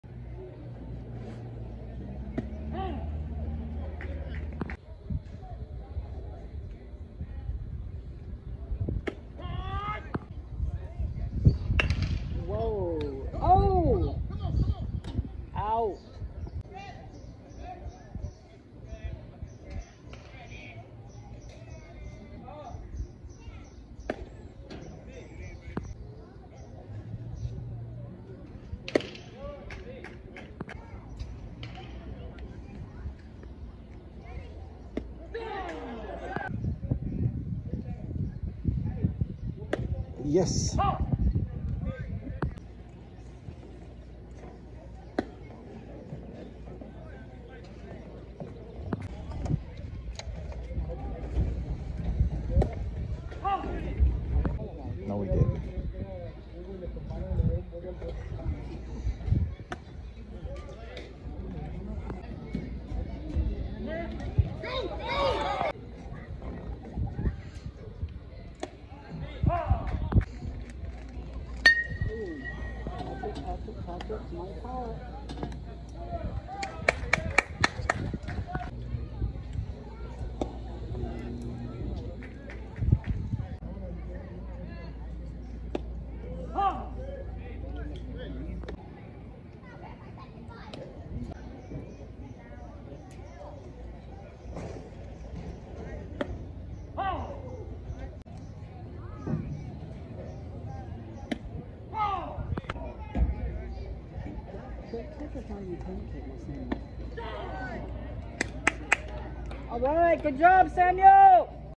Samuel pitching
Samuel won the player of the game